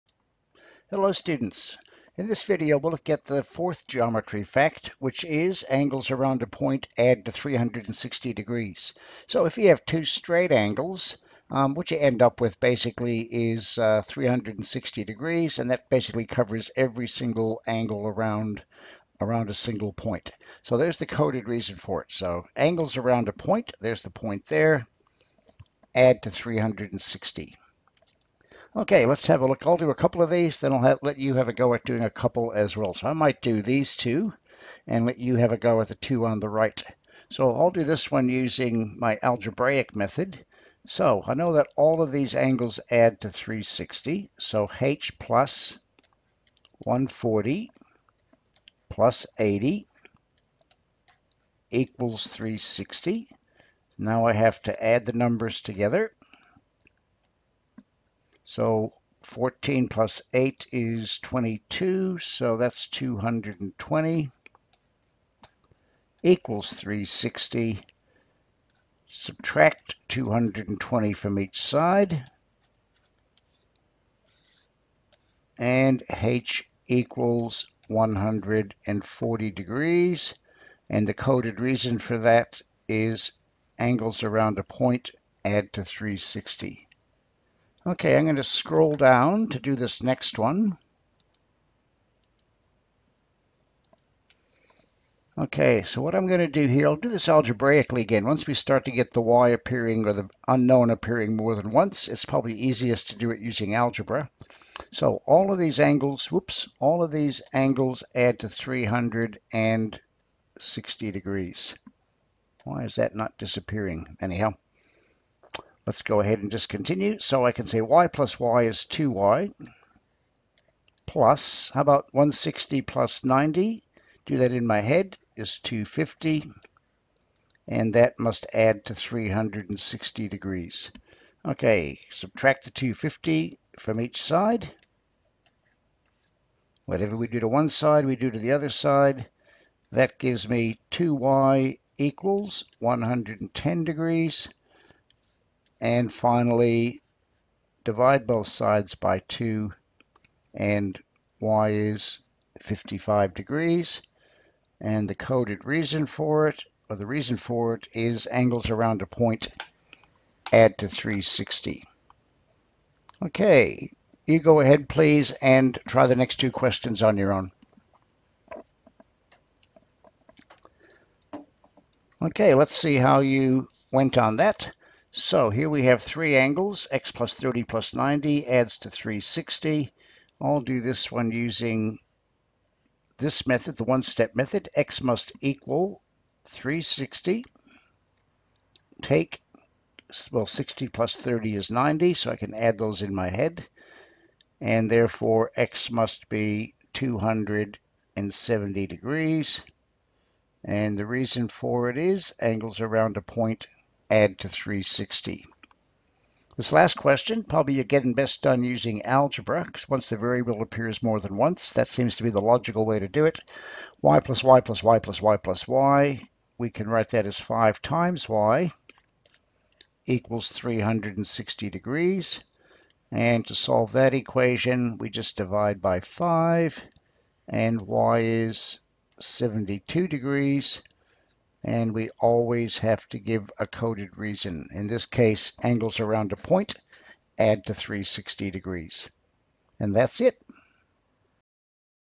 Geometry Fact #4: Angles Around a Point
Geometry Fact #4:  Angles around a point add to 360 degrees